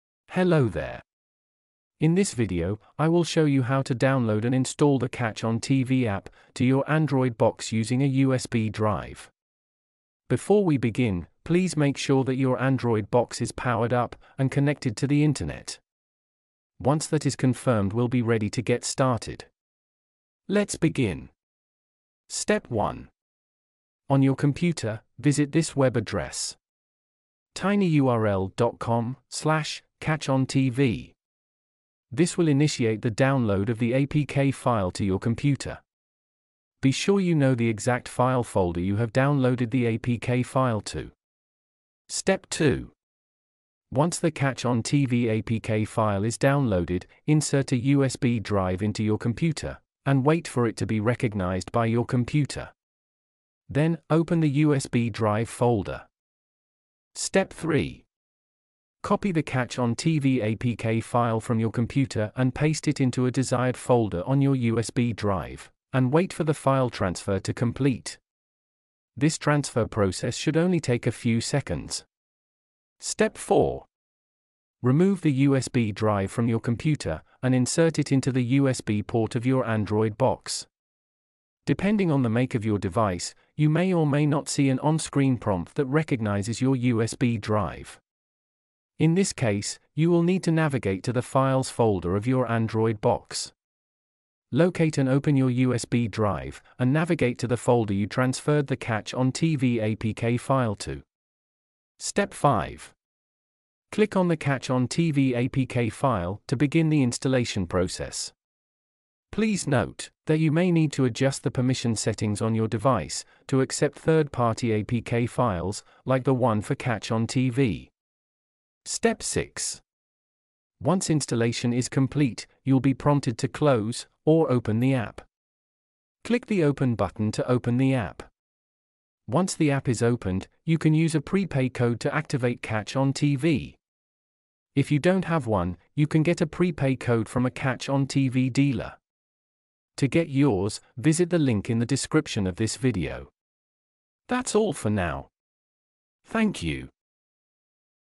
How to Install Catchon TV USB Drive to Android Box, Side Load Method
In this video, you will see how to download and install the CatchON TV app, to your android box using a USB Drive. Before we begin, please make sure that your android box is powered up, and connected to the internet.